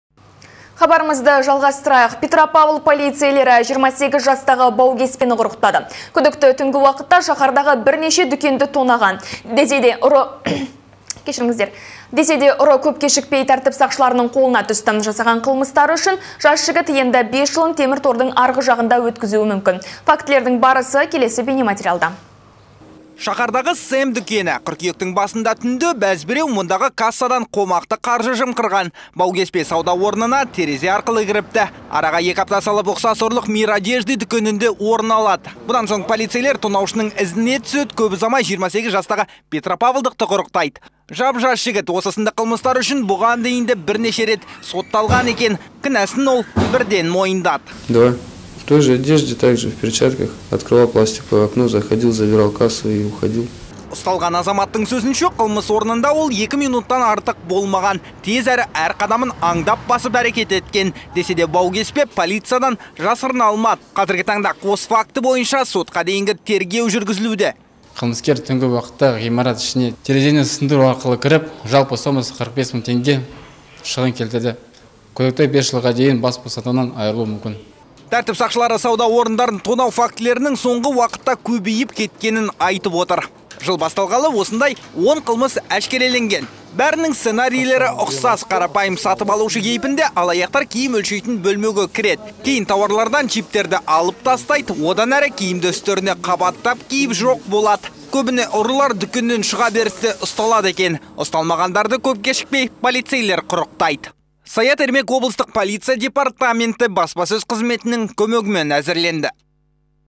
Полицейлер түнде дүкендерді тонаған баукеспені ұстады
Петропавл полицейлері 28 жастағы баукеспені құрықтады. Күдікті түнгі уақытта шаһардағы бірнеше дүкенді тонаған. Десе де, ұры көп кешікпей тәртіп сақшыларының қолына түсті. Жасаған қылмыстары үшін жас жігіт енді 5 жылын темір тордың арғы жағында өткізуі мүмкін. Фактілердің барысы келесі бейнематериалда.